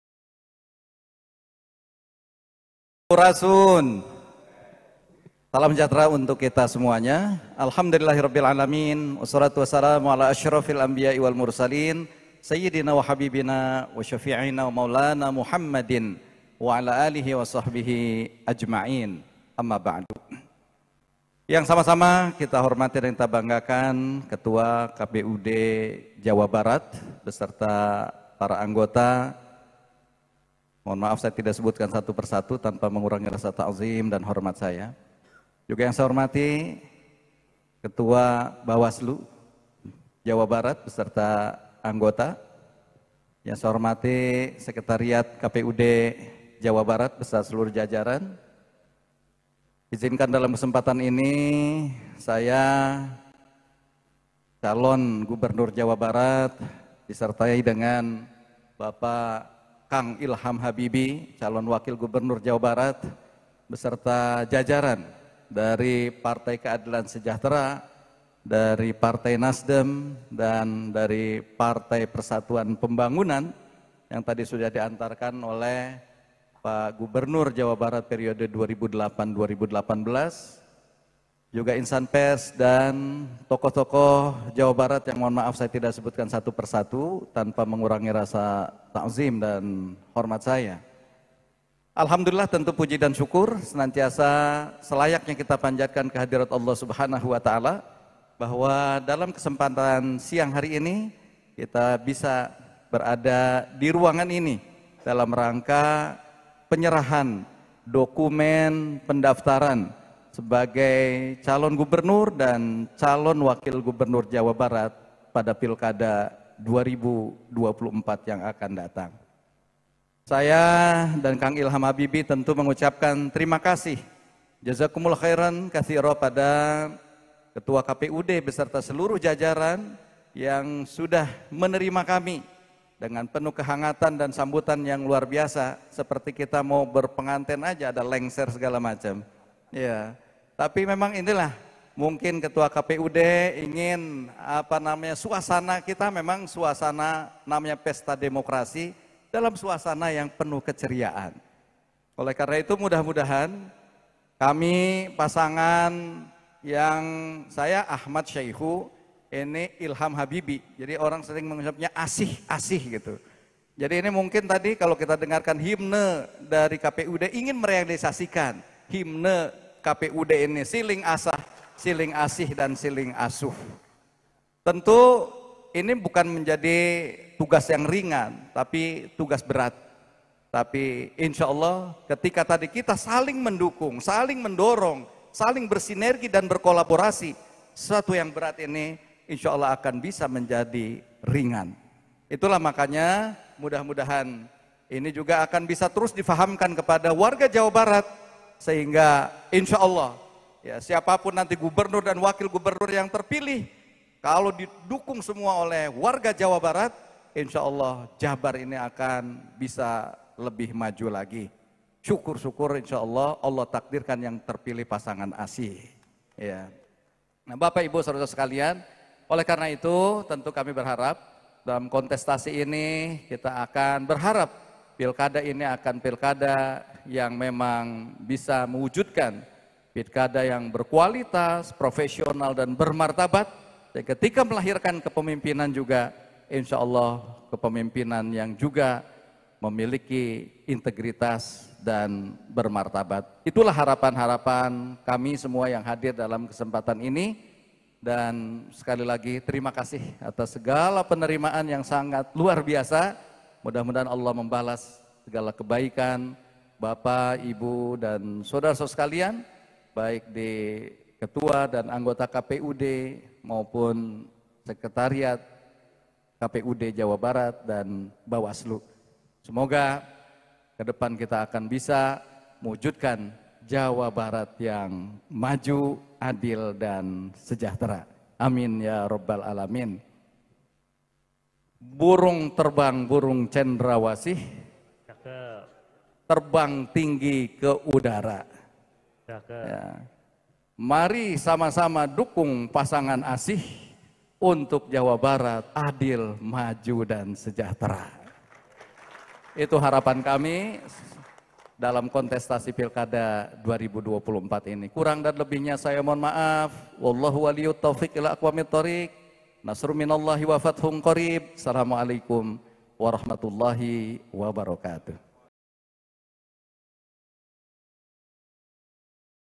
Pernyataan Ahmad Syaikhu Usai Registrasi KPU Pilkada Jabar 2024
Ahmad Syaikhu, calon Gubernur Jawa Barat dari Partai Keadilan Sejahtera (PKS), resmi mendaftarkan diri ke Komisi Pemilihan Umum (KPU) Jawa Barat pada Kamis (29/08/24).